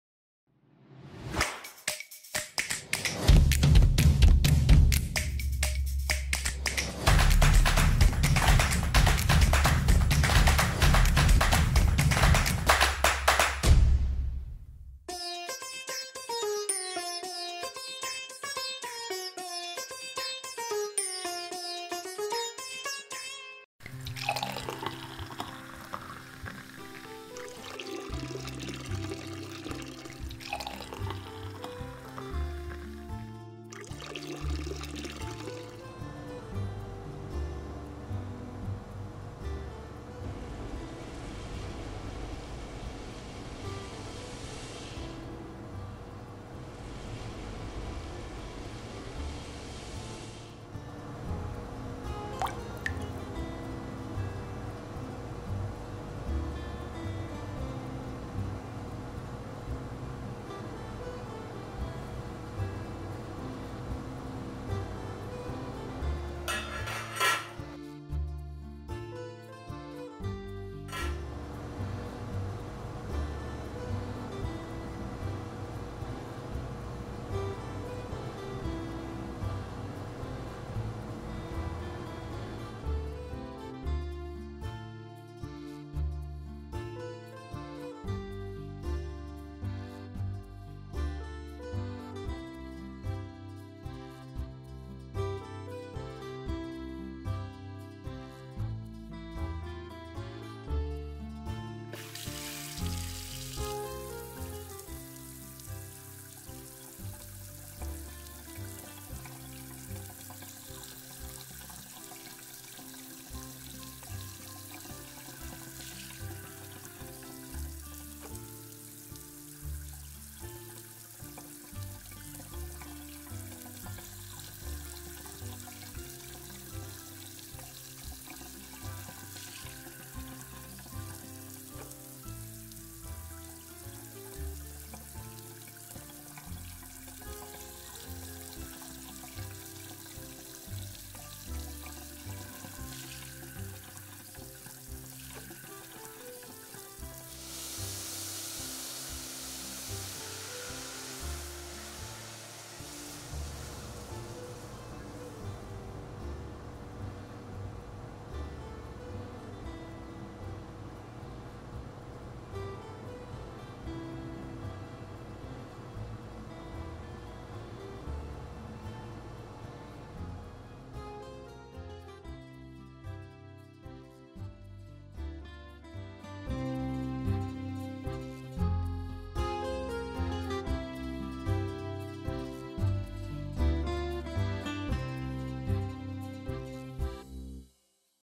Navratri halwa Recipe
The most commonly made dessert in Indian households, here is a simple yet yummy recipe of the Sooji Ka Halwa.
You can either enjoy it as is or have some garma garam puri's as accompaniment.

What you'll need:
1. Nourish Sooji
2. Sugar
3. Almonds
4. Pistachio
5. Desi Ghee
6. Cardamom
6. Zarda/ Food Color